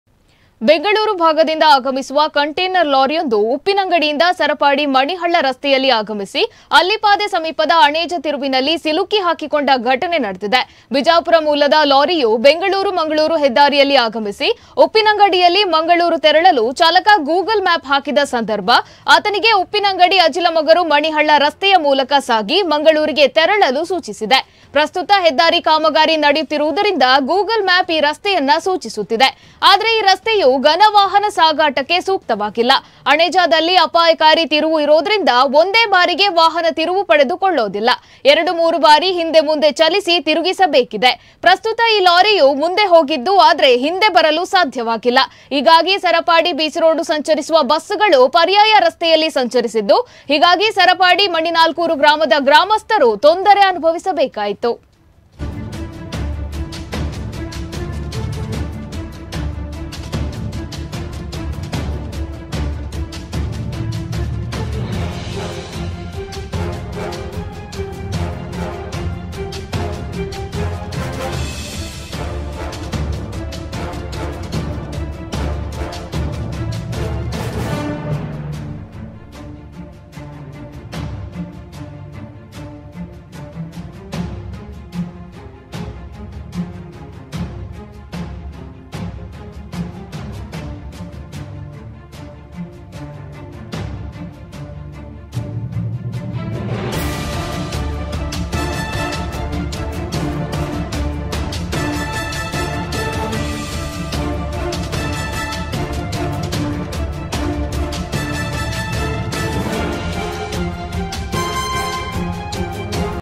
ಅಣೇಜ ತಿರುವಿನಲ್ಲಿ ಸಿಲುಕಿಹಾಕಿಕೊಂಡ ಕಂಟೈನರ್ ಲಾರಿ; ಬಸ್ ಇಲ್ಲದೆ ಮಣಿನಾಲ್ಕೂರು ಗ್ರಾಮದ ಗ್ರಾಮಸ್ಥರ ಪರದಾಟ!
ಘನವಾಹನಕ್ಕೆ ಸೂಕ್ತವಲ್ಲದ ದಾರಿ ತೋರಿಸಿದ ಗೂಗಲ್ ಮ್ಯಾಪ್!!!